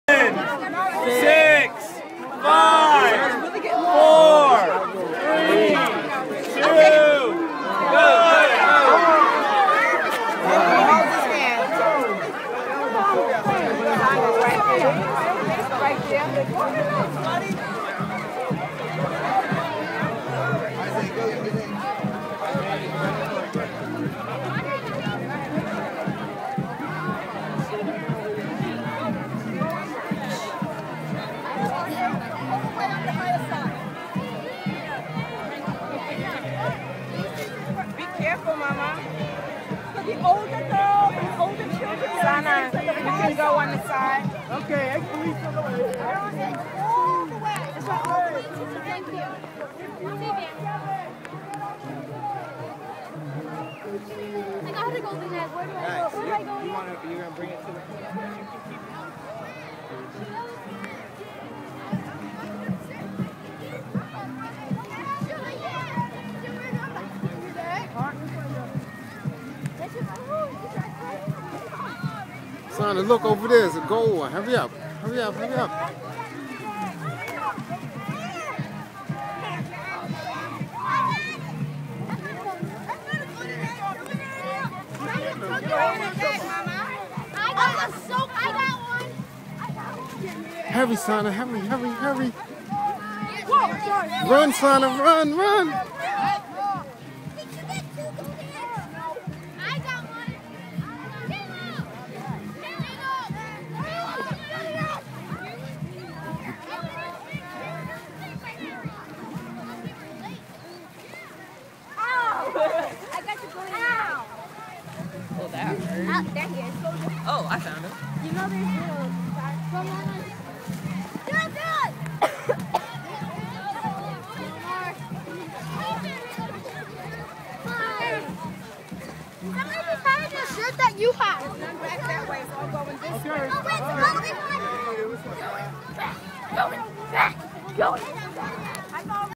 Issana Easter 2012
Issana Easter-Egg hunt 2012 Durham NC church.